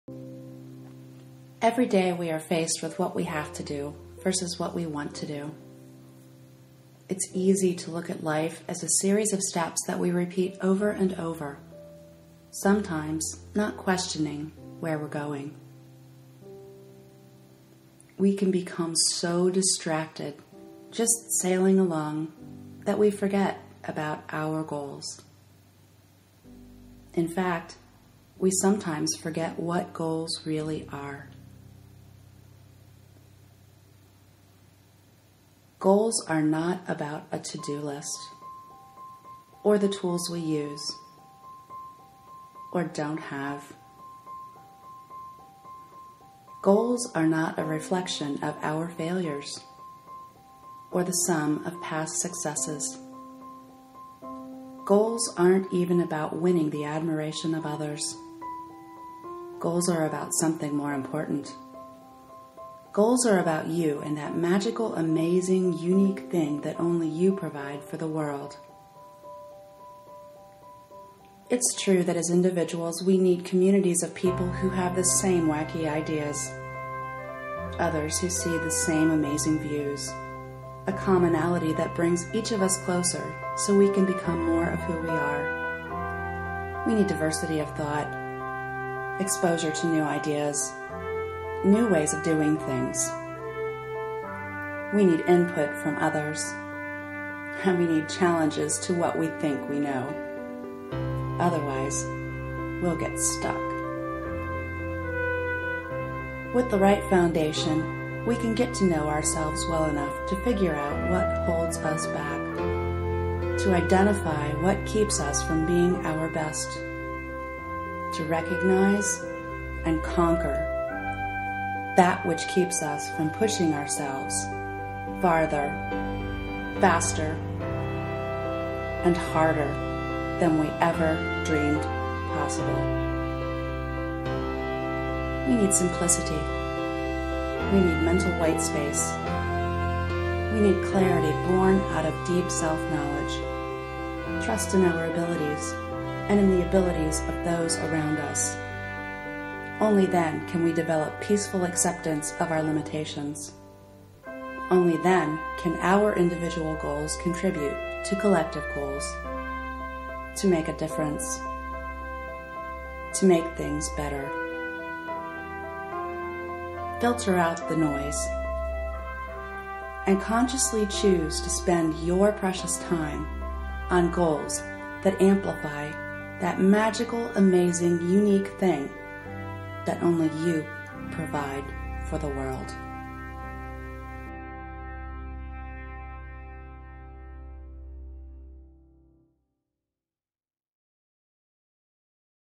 Goals
Prepared for an invited speaking engagement at the Marion Women's Business Council December 2014 meeting. Topic: Goals